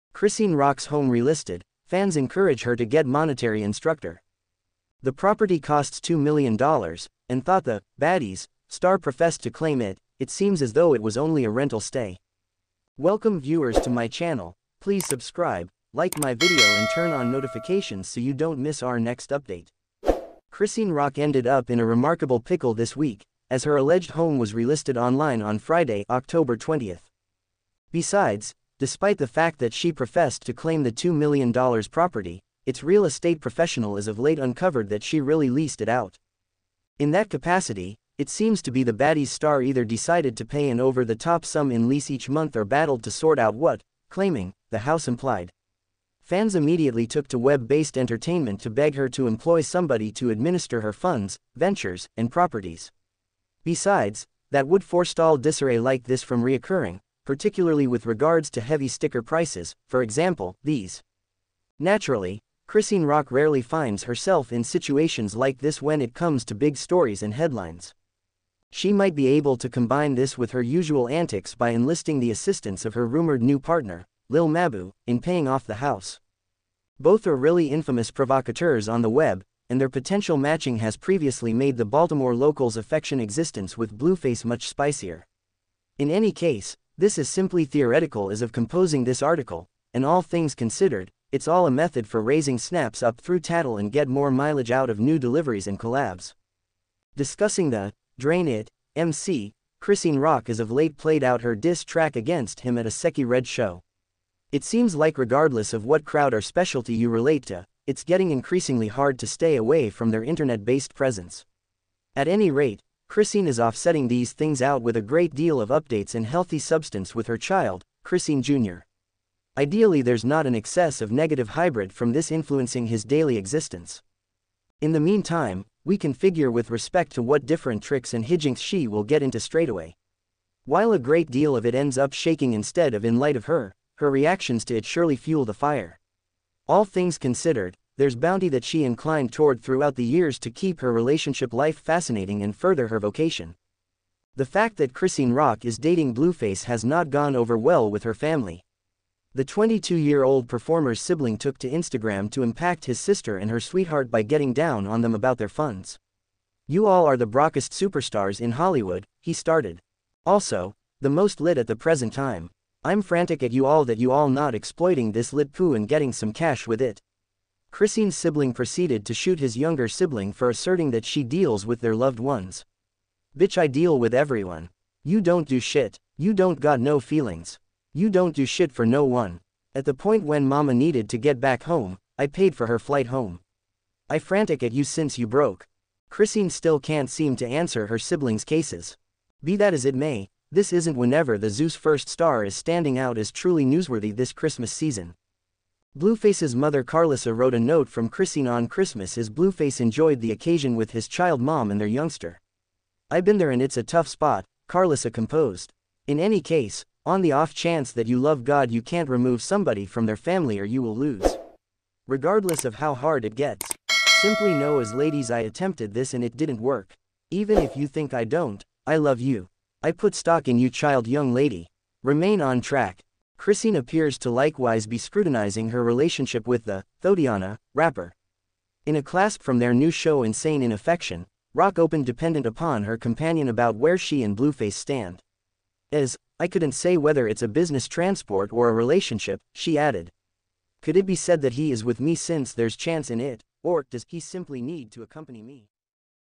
Blueface laughing at Chrisean Rock going broke after losing his House and Bank account
Chrisean Rock's Home Relisted, Fans Encourage Her To Get Monetary Instructor
The property costs $2 million, and thought the "Baddies" star professed to claim it, it seems as though it was only a rental stay.